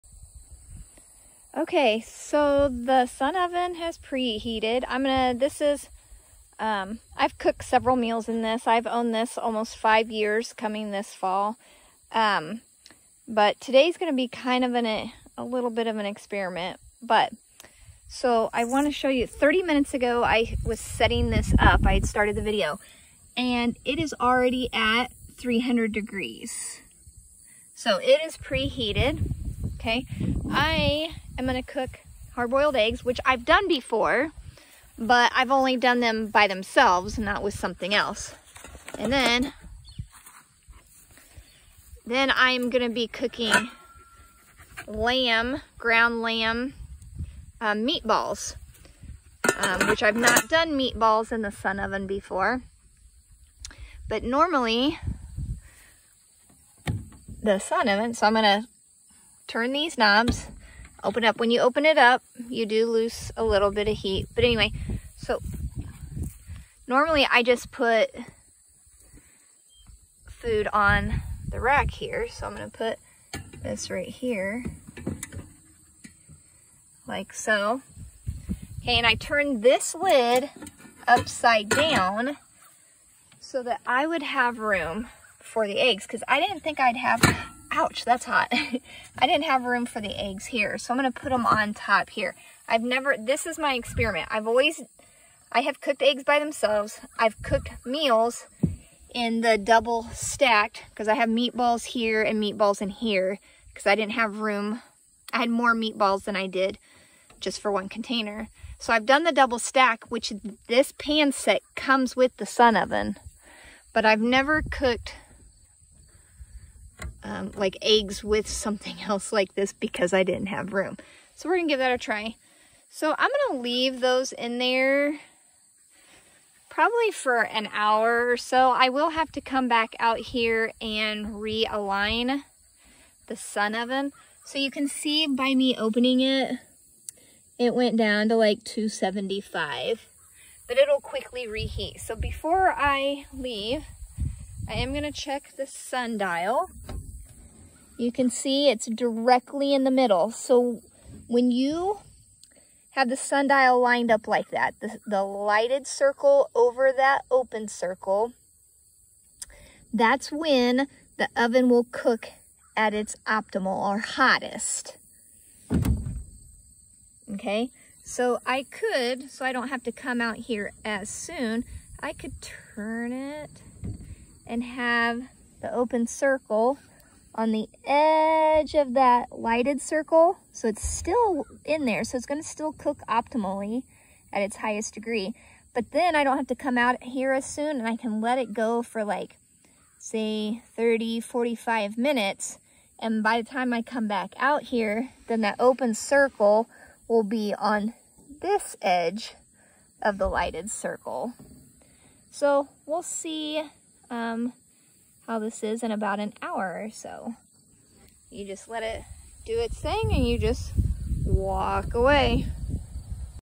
Sun oven meatballs and hard boiled eggs
I have owned this sun oven for 5 years and decided to start videoing meals I cook in it since there is an interest in using sun ovens or off grid cooking. I have baked hard boiled eggs in it before (by themselves) but never along side with other food. This is also my first time cooking meatballs in it. I have done meatloaf,  so same idea. So I guess you could say this is an experiment.  The meatballs are made from ground lamb.